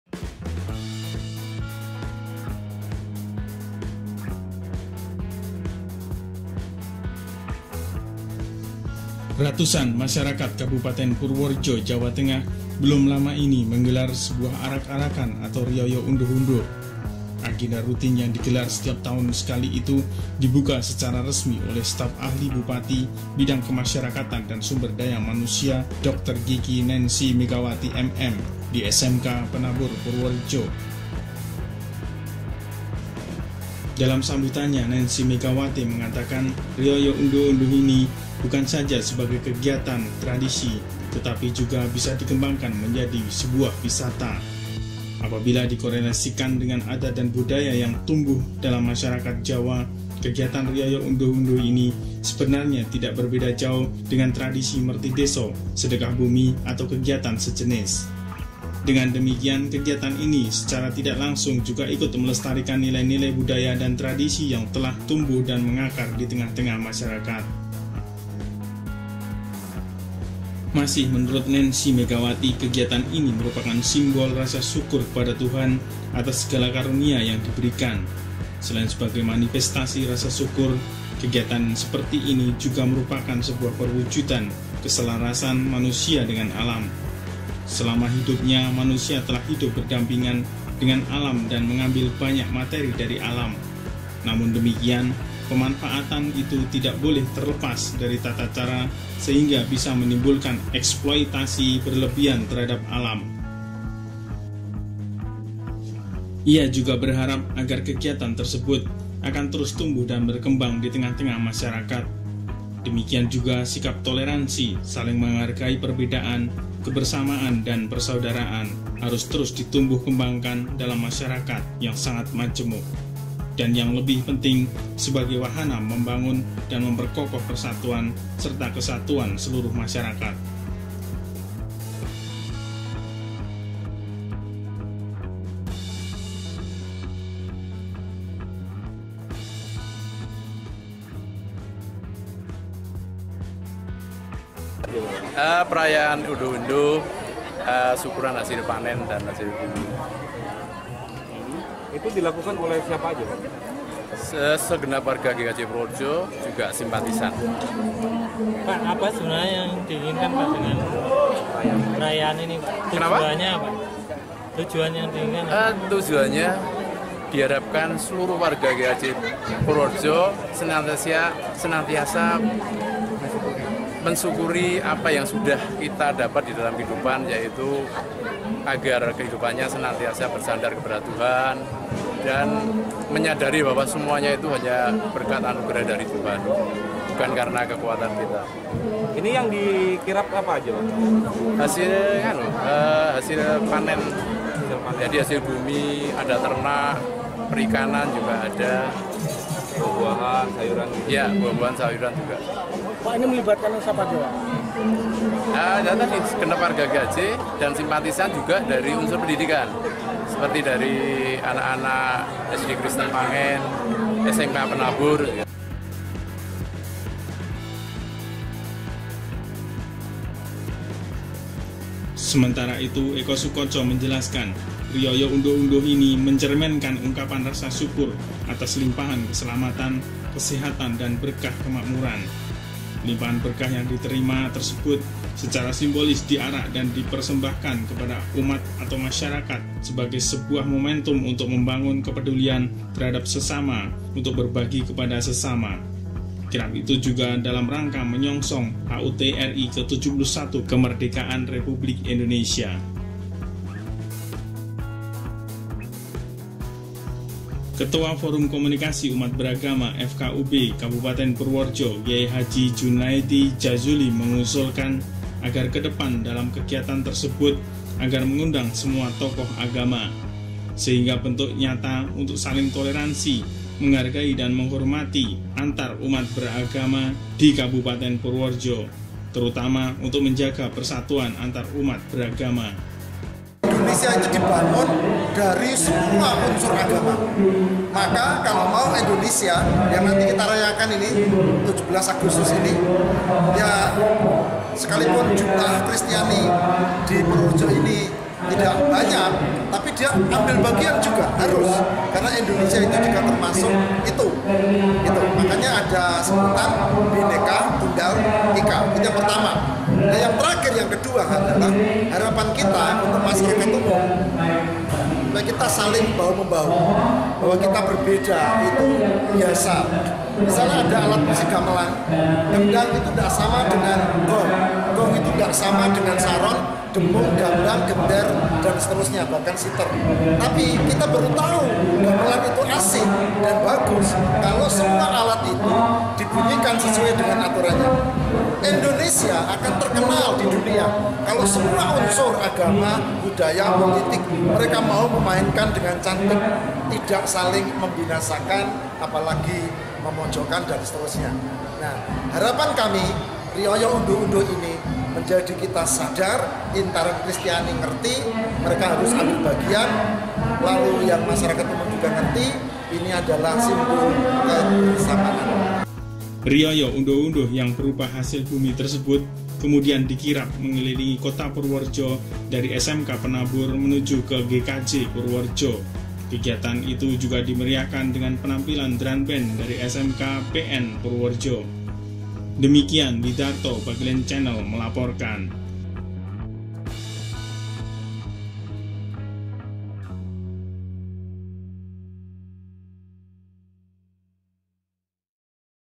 Undhuh Undhuh Ungkapan Rasa Syukur Atas Hasil Bumi
Ratusan warga masyarakat Kabupaten Purworejo melaksanakan kegiatan arak-arakan Riyaya Undhuh-undhuh. Agenda rutin yang digelar setiap setahun sekali itu, dibuka Secara Resmi oleh Staf Ahli Bupati bidang kemasyarakatan dan sumber daya manusia drg. Nancy Megawati, MM, di SMK Penabur Purworejo Sabtu (6/8/2016).